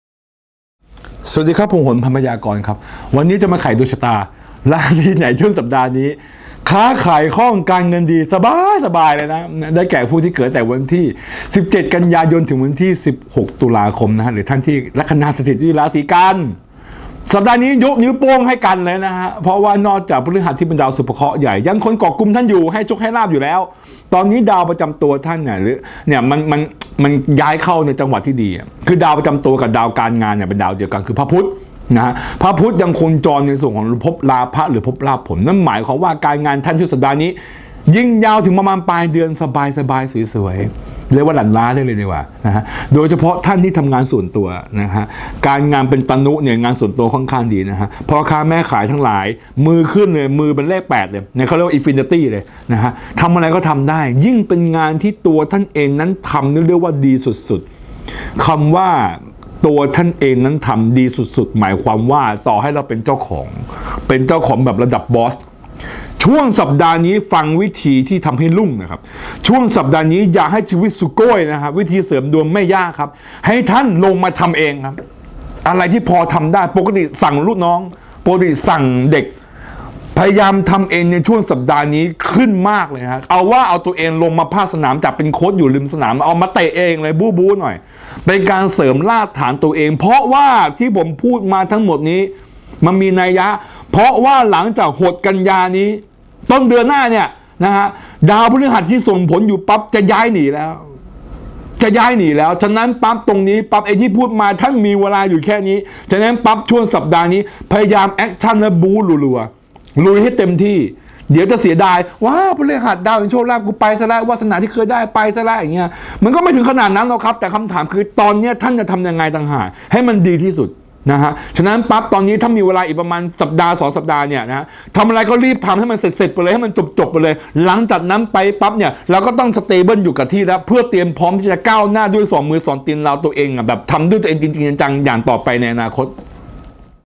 ดวงปัง 20-26 สิงหาคม 2560
ดวงปัง ๒๐ – ๒๖ ส.ค. ๒๕๖๐
ราศีดวงปังประจำสัปดาห์ การเงินดี การงานเฮง ค้าขายร่ำรวย
สัปดาห์นี้ราศีที่ดวงปังมาก เรียกได้ว่า ช่วงสัปดาห์นี้อะไรๆก็ดีไปหมด การเงินดี การงานเฮง ยิ่งค้าขายยิ่งร่ำรวยได้แก่ผู้ที่เกิดในลัคนา "ราศีกันย์" หรือ ผู้ที่เกิดในระหว่างวันที่ ๑๗ ก.ย. – ๑๖ ต.ค. เป็นช่วงที่ดีมากๆโดยเฉพาะผู้ที่ประกอบอาชีพ พ่อค้า แม่ค้า ดวงการค้าขายจะดียาวไปจนถึงปลายเดือนเลยทีเดียว บอกได้เลยว่าเดือนนี้ท่านควรจะต้องรีบกอบโกยอะไรที่ทำแล้วดีให้รีบทำ เพราะต้นเดือนหน้าดาวพฤหัสจะย้าย ดังนั้นตอนนี้คิดอะไรได้ให้รีบทำไว้ก่อนจะได้ไม่นึกเสียดายทีหลัง ส่วนเคล็ดลับวิธีเสริมดวงแบบละเอียดๆในเรื่องต่างๆต้องไปฟังจากโหรพันพยากรณ์ต่อกันเลย.